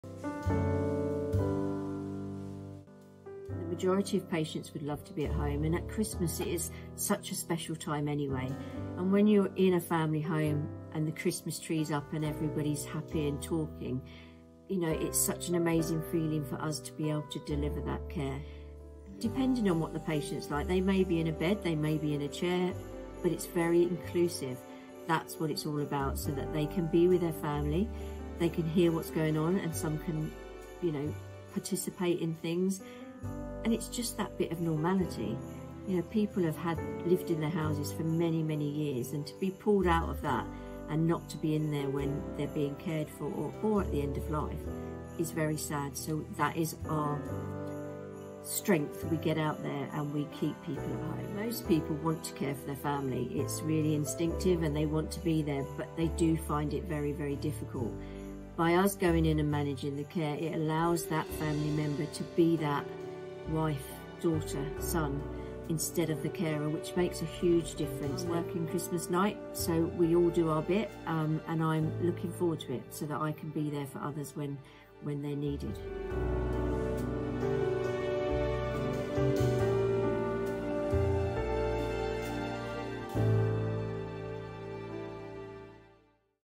Home for Christmas
Jackie explains how your support will help Island families stay together this Christmas.